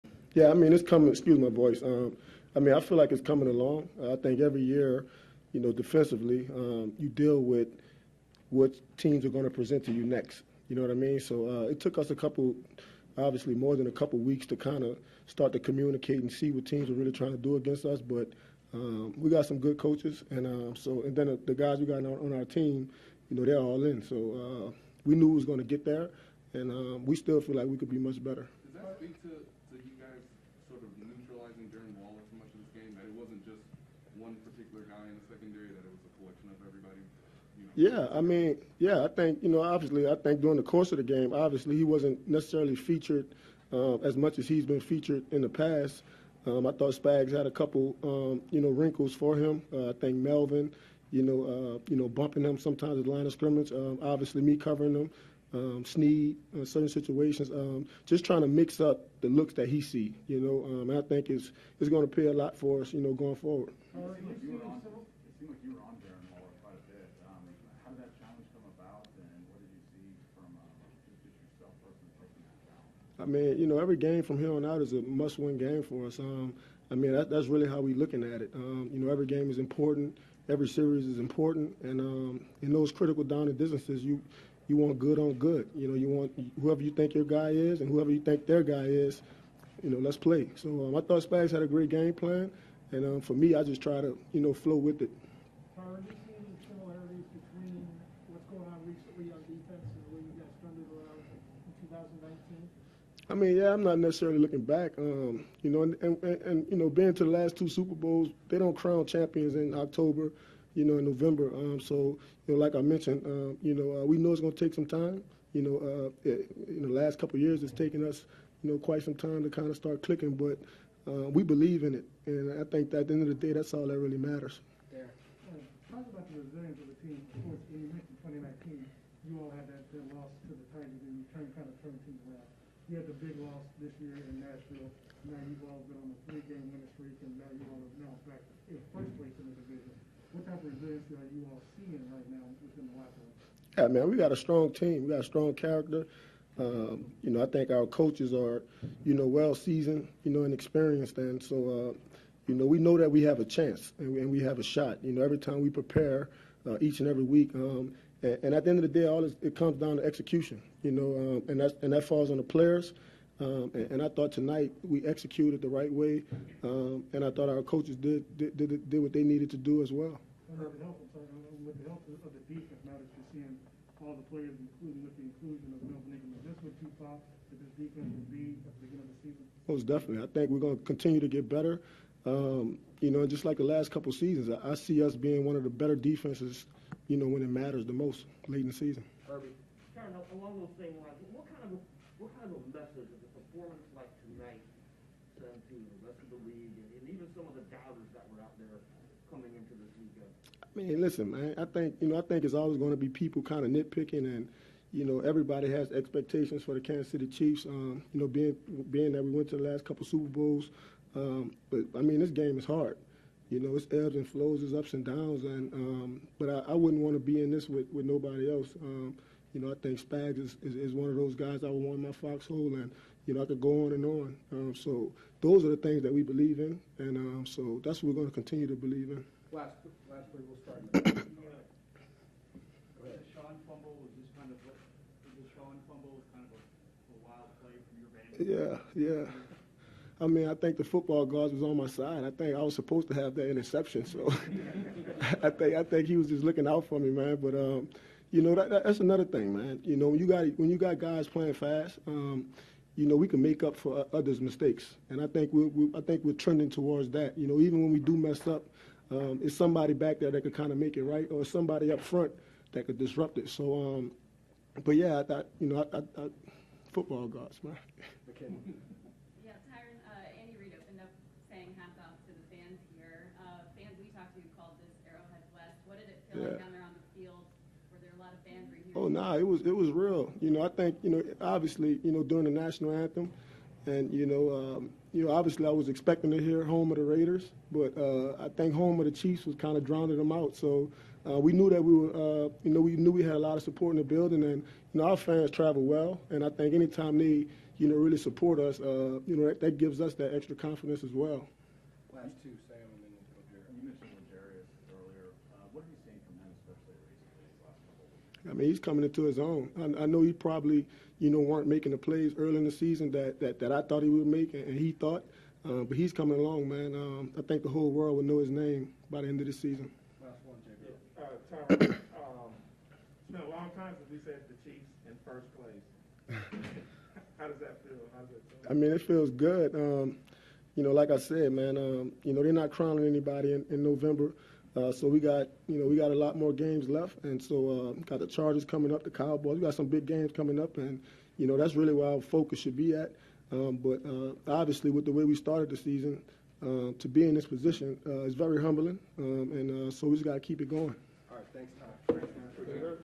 Tyrann Mathieu: “I wouldn’t want to be in this with anybody else" | Week 10 Press Conference
S Tyrann Mathieu speaks to the media after win over the Las Vegas Raiders.